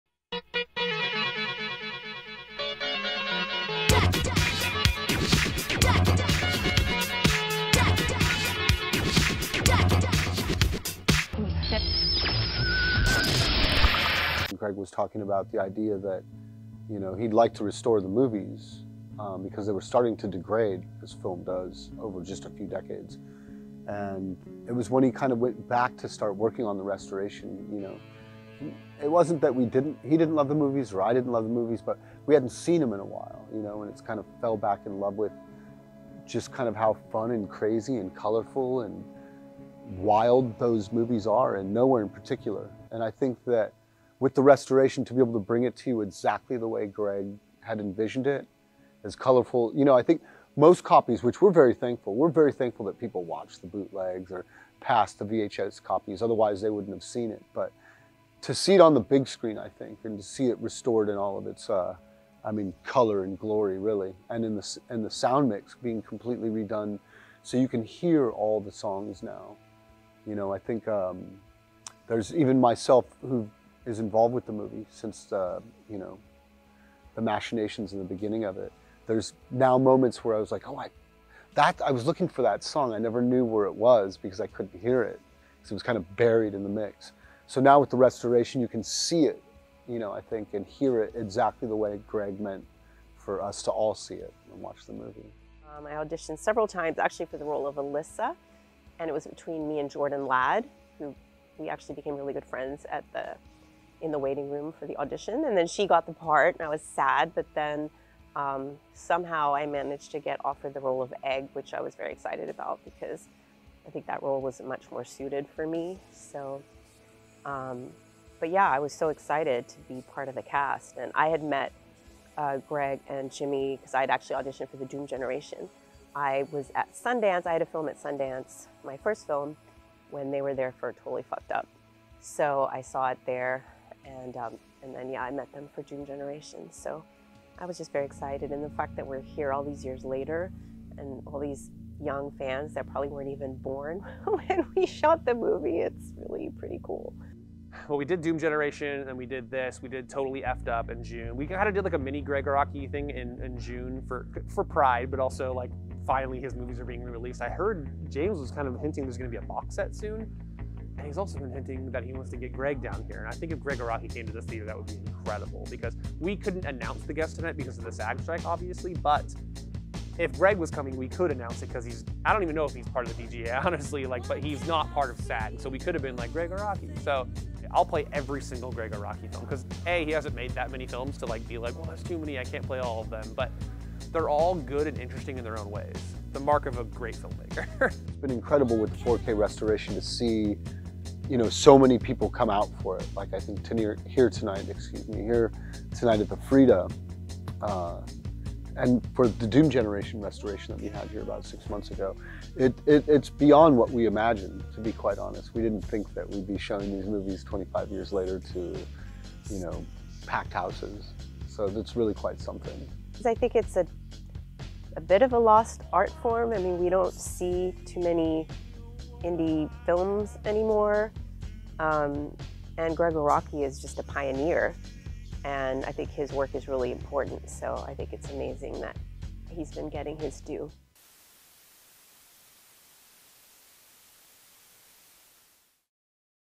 “Nowhere” is finally “somewhere” in theaters
The 1997 indie film “Nowhere” finally hit theaters in all its wildly colorful glory on Saturday, Nov. 4, after more than 25 years of being a pirated gem for audiences.